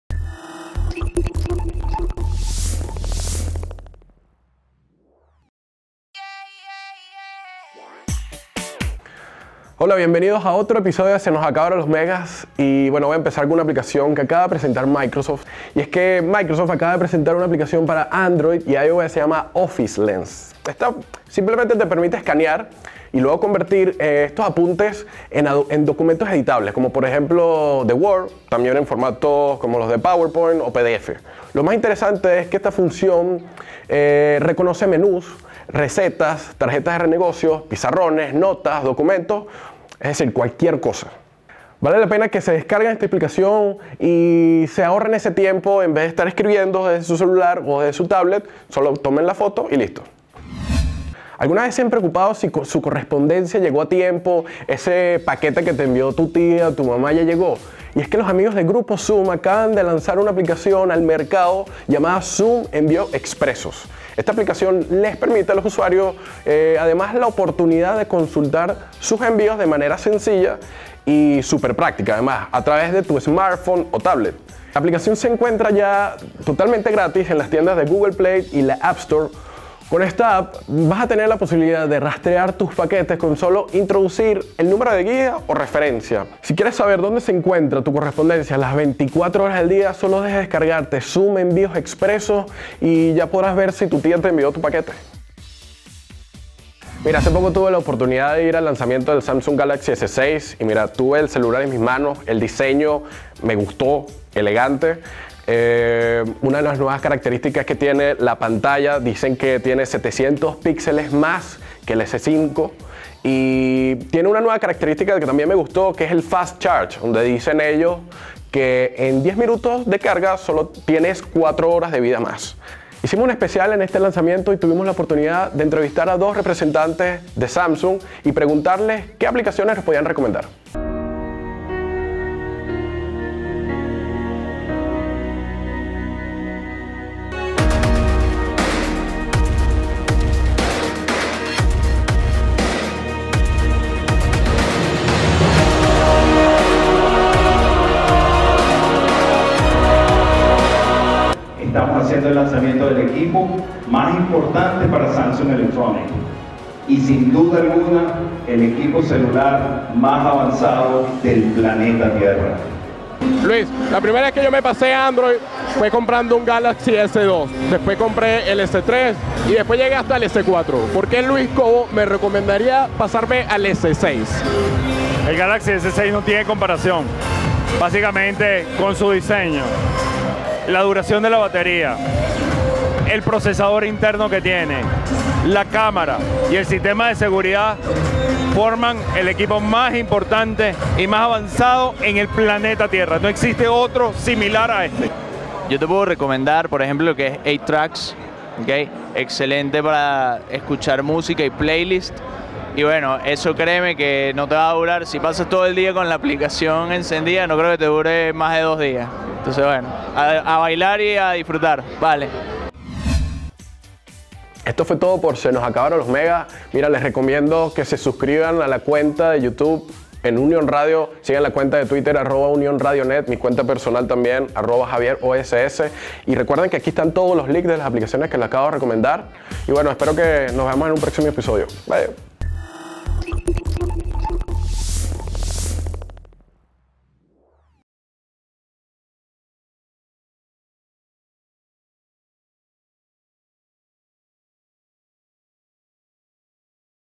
Se nos acabaron los megas | Edición Especial
En este episodio de Se nos acabaron los megas, @JavieRoss nos habla de 2  aplicaciones indispensables para tu smartphone. Además de mostrarnos como fue el lanzamientos del Samsung Galaxy S6.

Links de App
Offices Lens